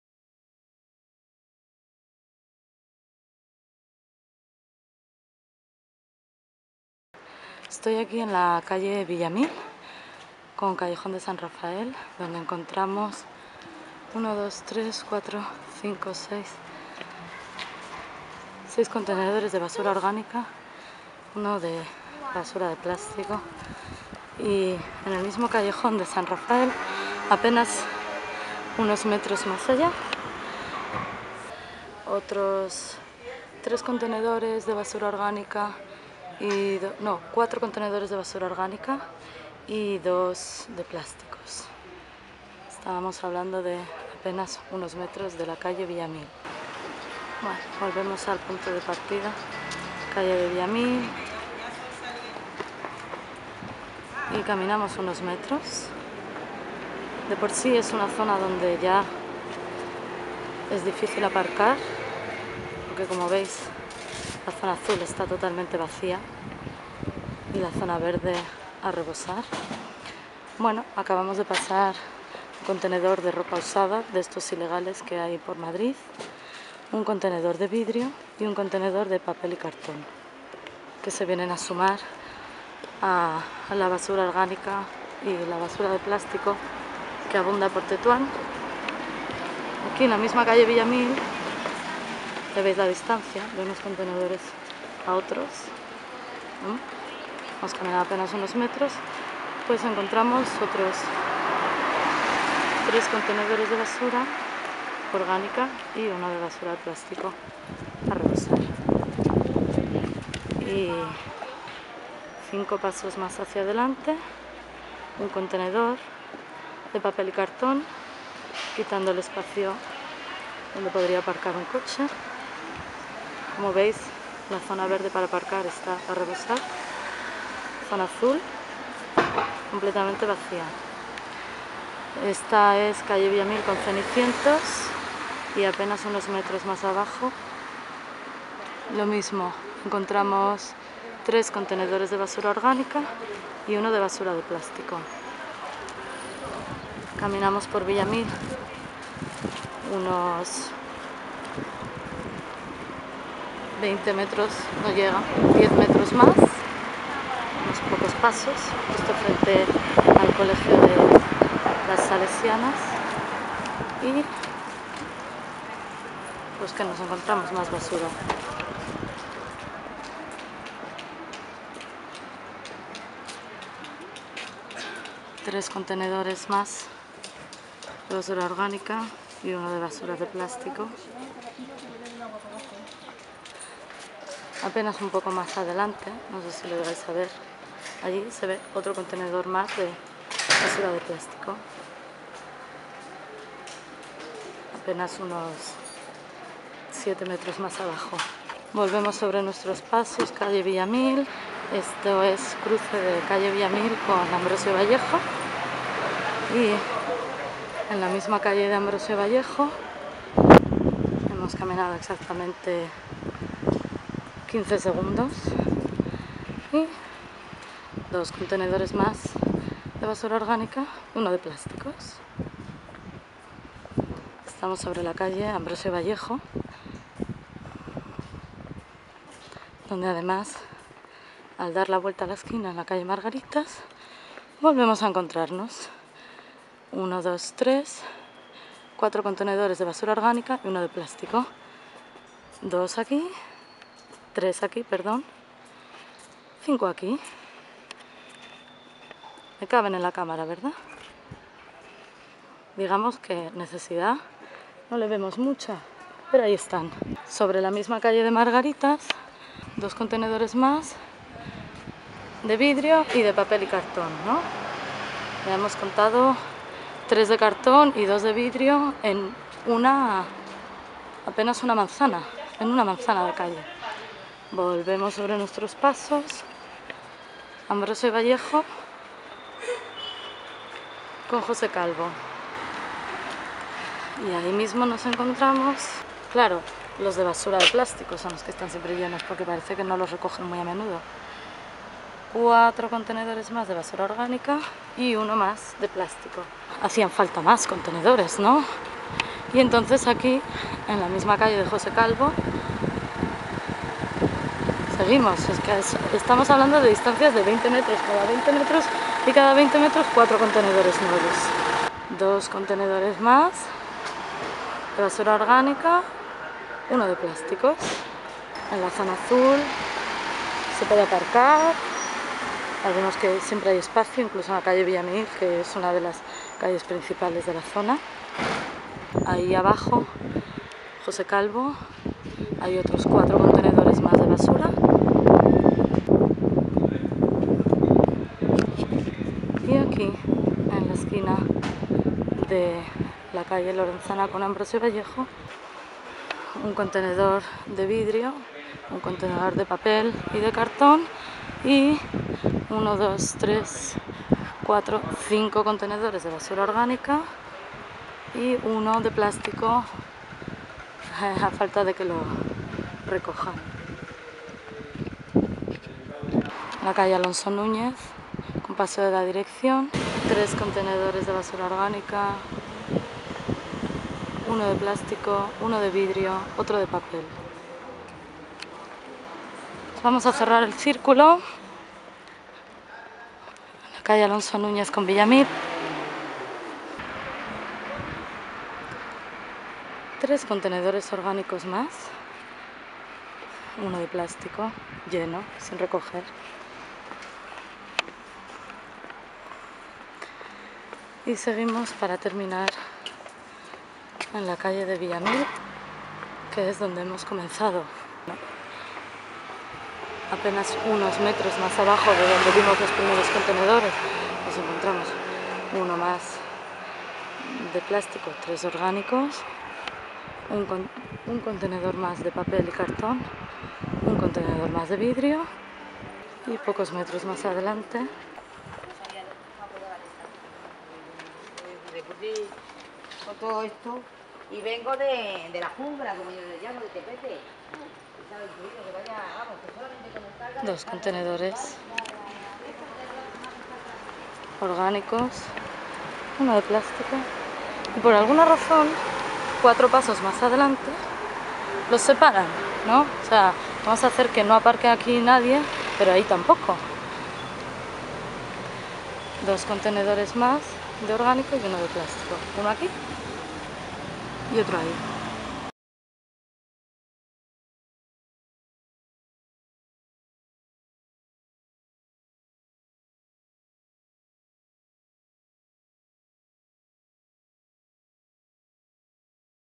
Ahogados por la basura en Tetuán
Paseamos por el barrio de Berruguete del Distrito de Tetuán y descubrimos la altísima densidad de contenedores de basura. En algunas calles encontramos un contenedor cada 6 metros. ¿Se necesitan tantos contenedores?